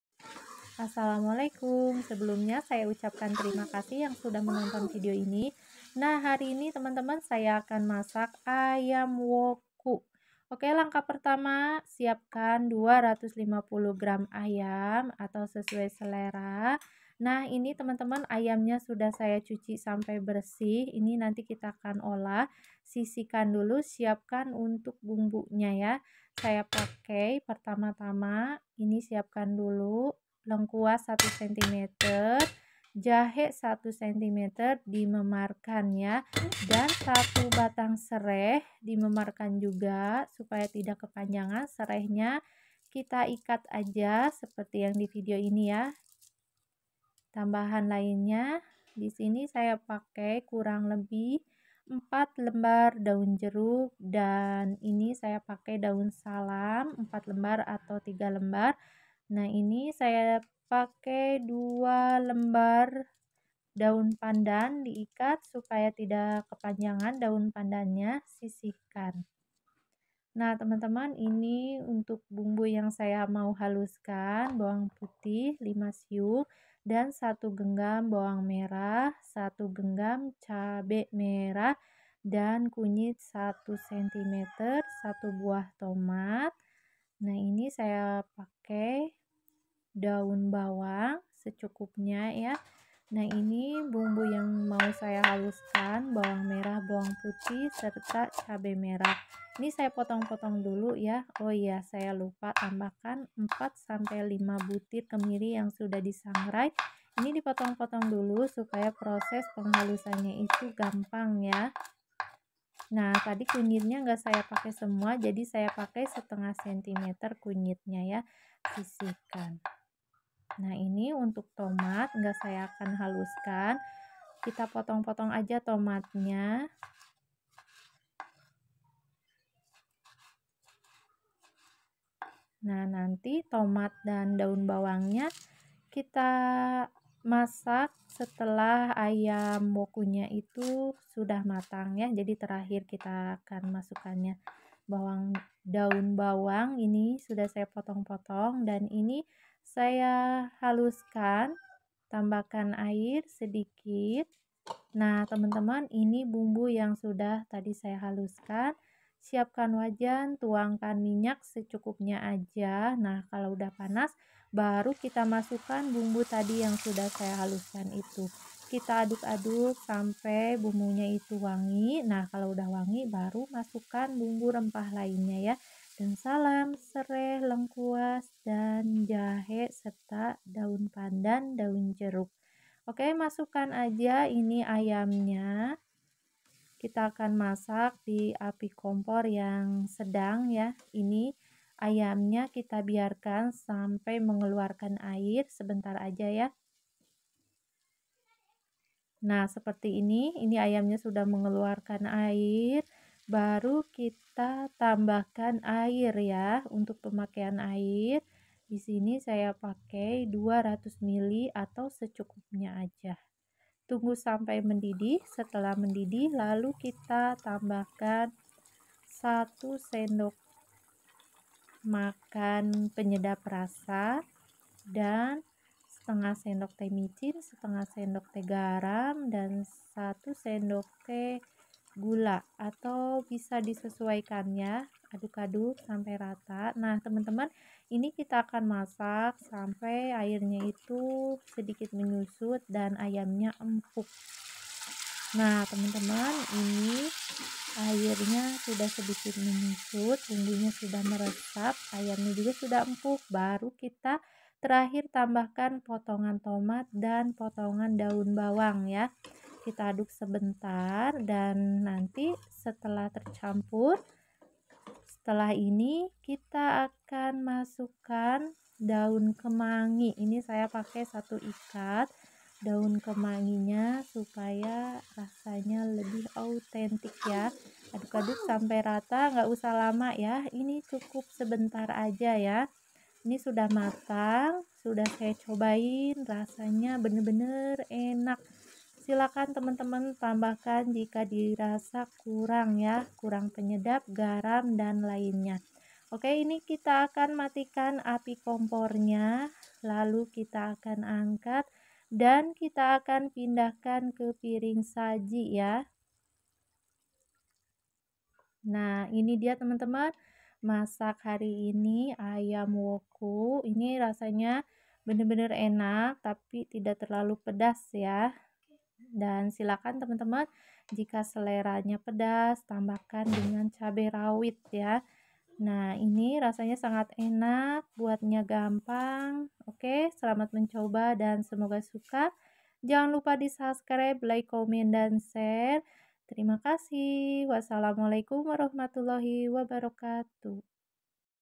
TERNYATA MASAK AYAM SEPERTI INI SANGAT !! RASANYA ENAK AYAM WOKU
Ayam woku resep masakan rasanya sangat enak menu lauk yang bikin boros nasi @ani elvina

Bahan

250g ayam

1 cm lengkuas
1 cm jahe
4 lembar daun jeruk
4 lembar daun salam
1 batang sereh
2 lembar daun pandan

5 siung bawang putih
1 genggam bawang merah
1 genggam cabe merah
1/2 cm kunyit
4 butir kemiri

200ml air

Minyak secukupnya

1 sdm penyedap rasa
1/2 sdt micin
1/2 sdt garam
1 sdt gula

1 ikat daun kemangi

Selamat mencoba semoga suka ❤️